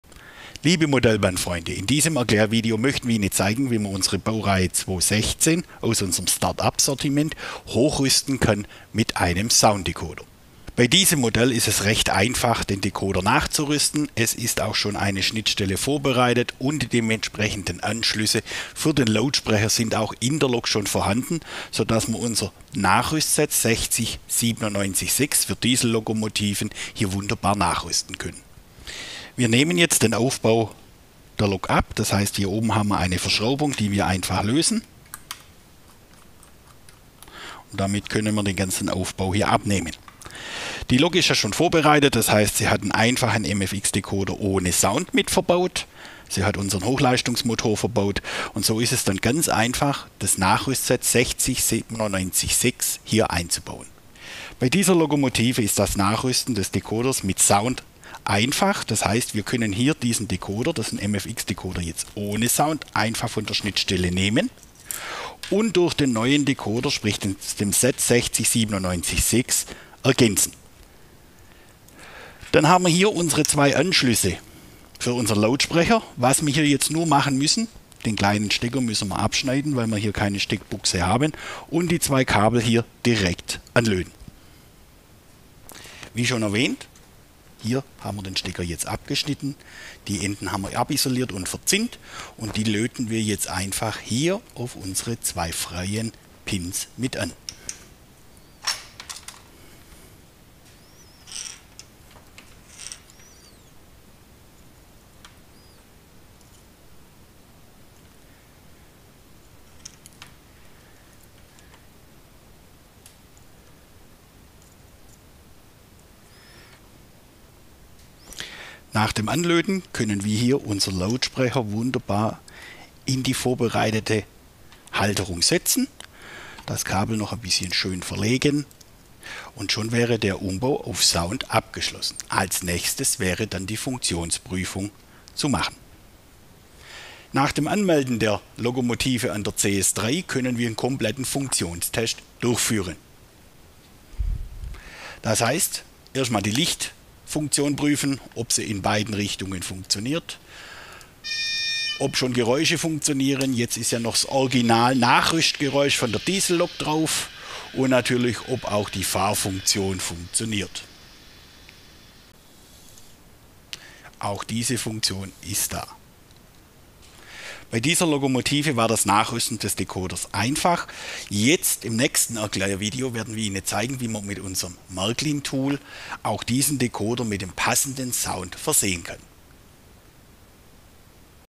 Sounddecoder bei Märklin Start Up Lokomotive einbauen / mSD3 mit Baureihe 216 (36218)
In diesem Erklärvideo zeigen wir euch, wie man den Sound einer Märklin Start Up Lokomotive mit einem Sounddecoder toll verbessern kann. Wir zeigen den kompletten Einbau und Umbau in einfachen Schritten, die man bequem zu Hause nachbauen kann. Wir nutzen den Sounddecoder mSD3 (Artikelnummer #60976) und die Märklin Start Up Diesellokomotive Baureihe 216 (Artikelnummer #36218).

Märklin Start Up Diesellokomotive Baureihe 216